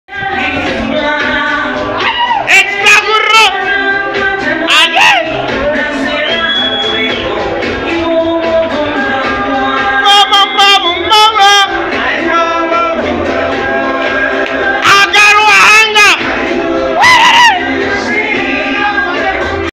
Dj Gideon Uganda riddim star deejays
Dj GIDEON Ug mixing live Riddim Star deejays